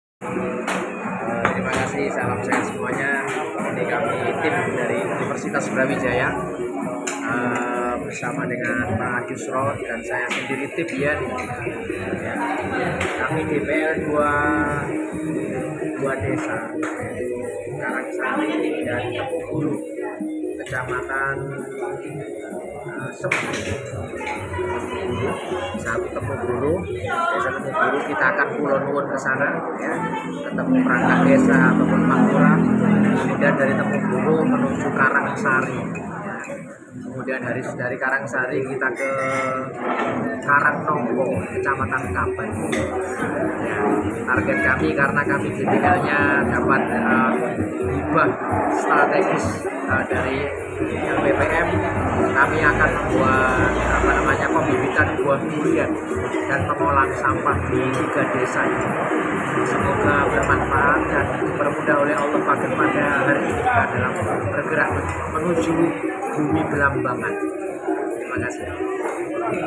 Dr Tibyan dan Tim UB BDS Sarapan menuju lokasi Desa Desa di Kabupaten Banyuwangi MMD 1000 Desa Jatim
Survei Mini Broadcast center Desa Kalipuro Kab Banyuwangi MMD-BDS 1000 Desa Jatim minggu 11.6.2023